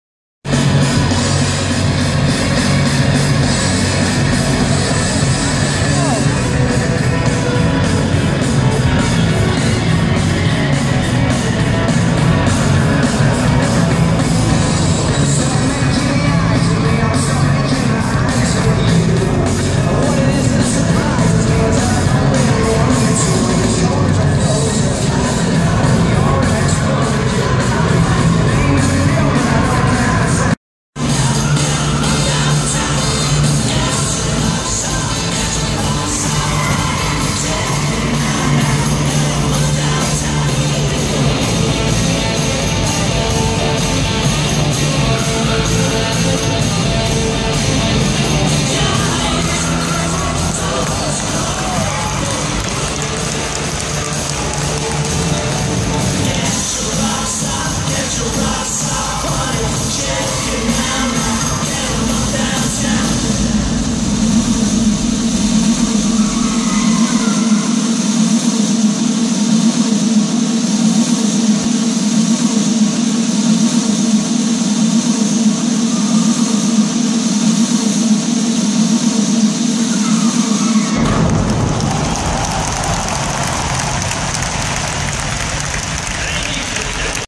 Top Gear Live 2013
Top Gear Live 2013 na Stadionie Narodowym w Warszawie.